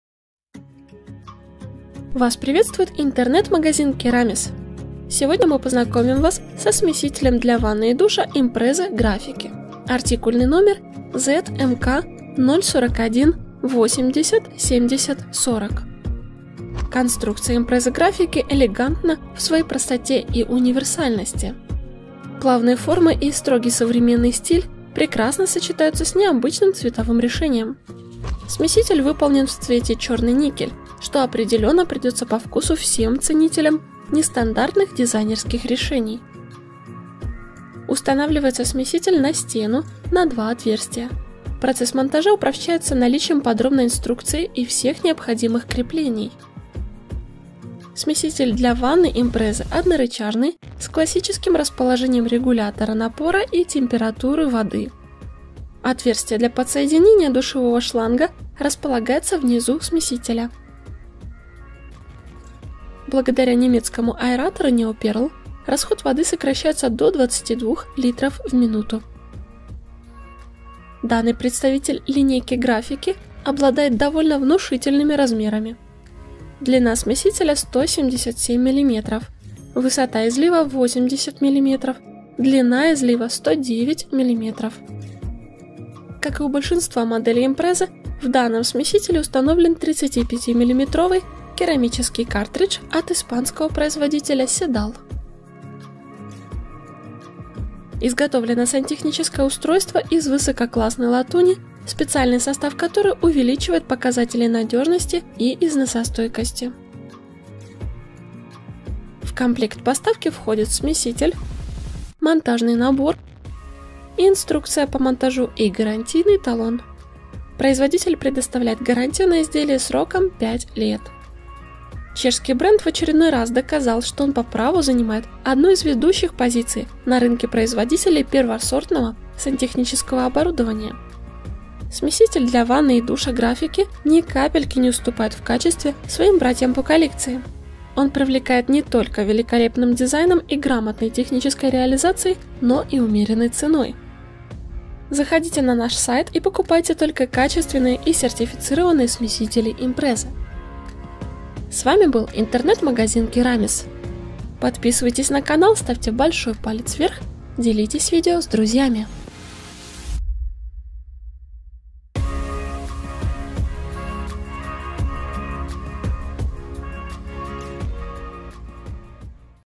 Классический лофт в вашей ванной. Смеситель Imprese Grafiky ZMK041807040
Узнать цену:
Конструкция Imprese Grafiky элегантна в своей простоте и универсальности. Плавные формы и строгий современный стиль прекрасно сочетаются с необычным цветовым решением. Смеситель выполнен в цвете "черный никель", что определенно придется по вкусу всем ценителям нестандартных дизайнерских решений.
Изготовлено сантехническое устройство из высококлассной латуни, специальный состав которой увеличивает показатели надежности и износостойкости.
Смеситель для ванны Imprese - однорычажный с классическим расположением регулятора напора и температуры воды. Отверстие для подсоединение душевого шланга располагается внизу смесителя.
Устанавливается смеситель на стену, на два отверстия. Процесс монтажа упрощается наличием подробной инструкции и всех необходимых креплений.
Подключение к системе водоснабжения осуществляется при помощи гибких шлангов под диаметр 1/2 G.
Краткие характеристики смесителя Imprese Grafiky ZMK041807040
- Страна производитель
- Высота излива: 80 мм;
- Конструкция: неповоротный излив;
- Цвет: черный никель;
- Способ монтажа: на стену, вид монтажа – наружный;
- Управление: однорычажный;
- Гарантия: 5 лет.
В комплект поставки входит:
- гибкие шланги подвода воды;
- монтажный набор, который включает в себя все необходимое для быстрого монтажа;
- точная инструкция по монтажу и гарантийный талон;
- срок гарантии на изделие составляет 5 лет;